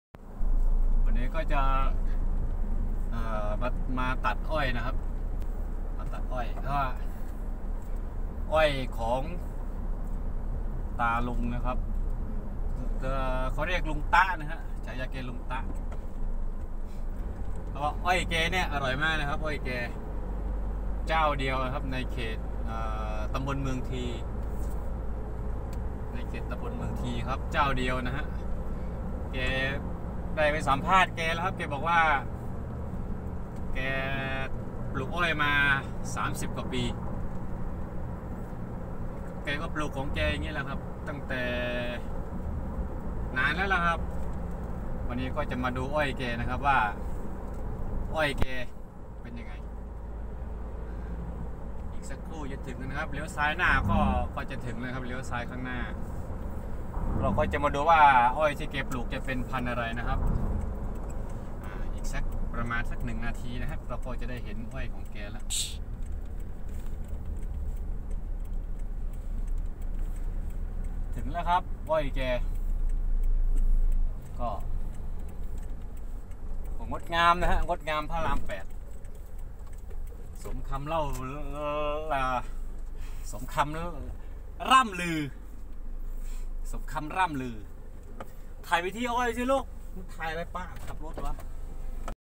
ตามหาอ้อยในตำนาน
สุดยอดอ้อยคั้นน้ำแห่งเมืองที